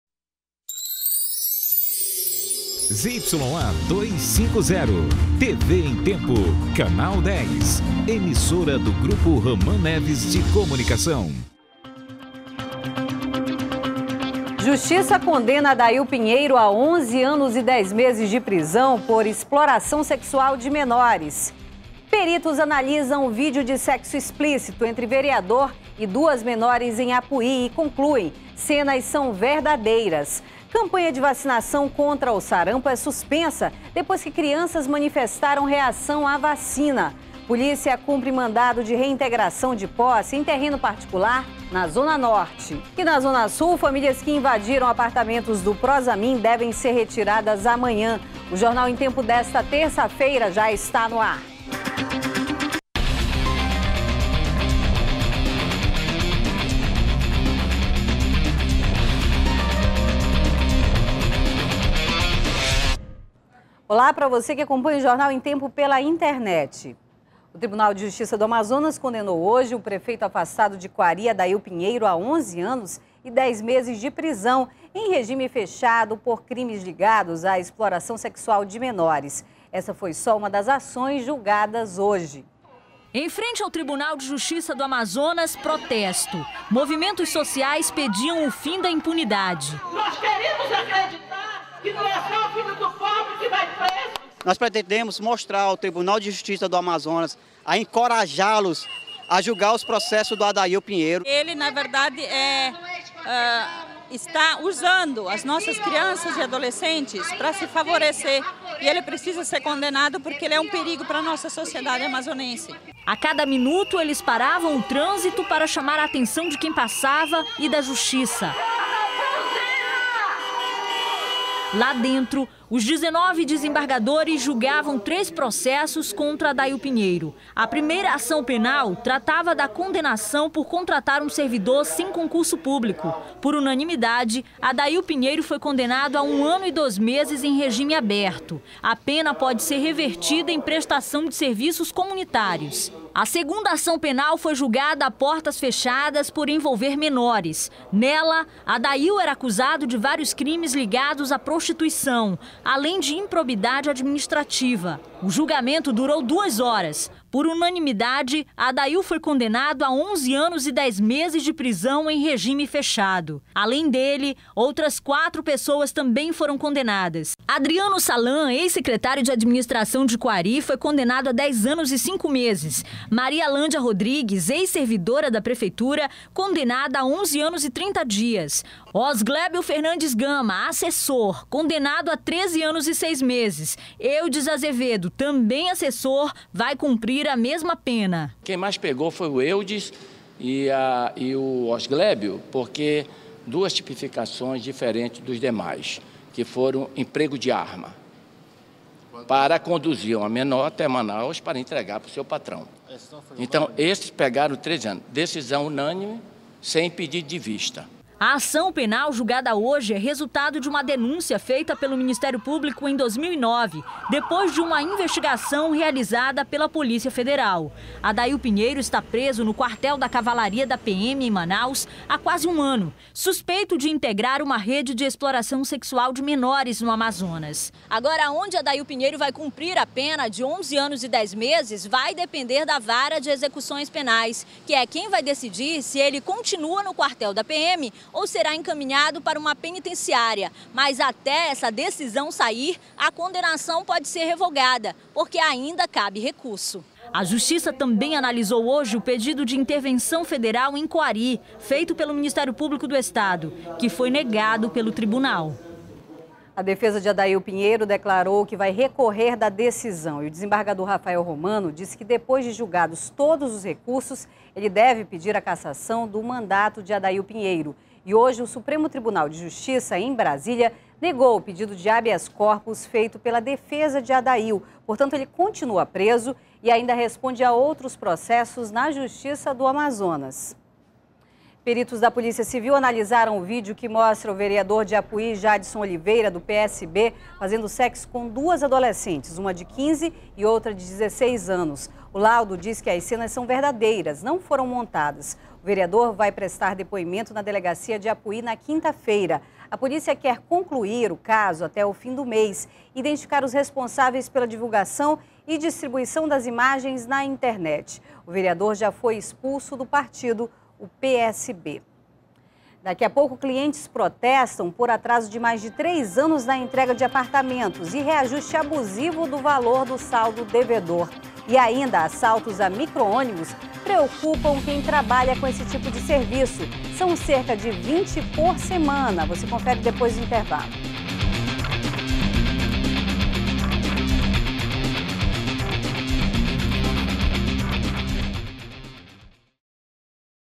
Adail Pinheiro é condenado a 11 anos e 10 meses de prisão em regime fechado
01º BLOCO JORNAL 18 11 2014 - Adail Pinheiro é condenado a 11 anos e 10 meses de prisão em regime fechado